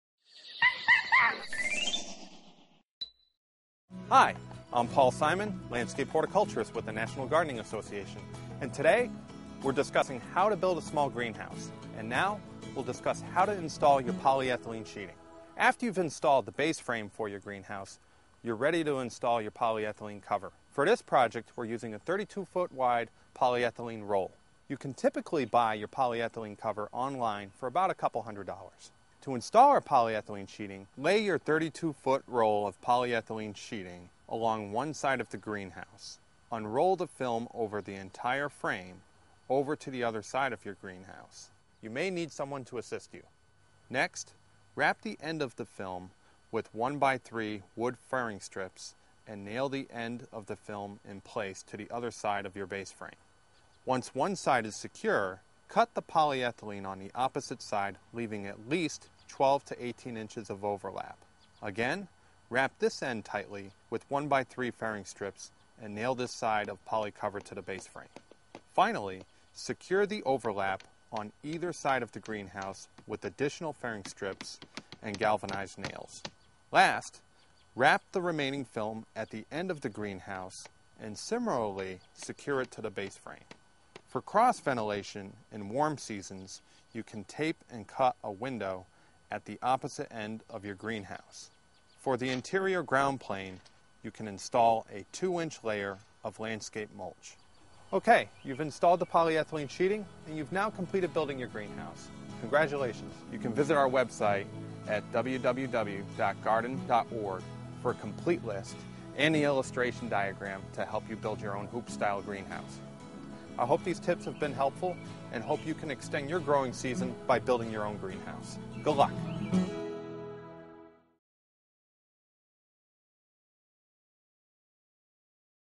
Building a Greenhouse - Installing the Polyethylene Sheeting
Paul Simon, Landscape Horticulturist with the National Gardening Association, demonstrates how to build your own small hoop-style greenhouse. Tips include advice on purchasing greenhouse supplies, choosing a location for your greenhouse, and guidance on how to construct a small 14ft x 20ft greenhouse.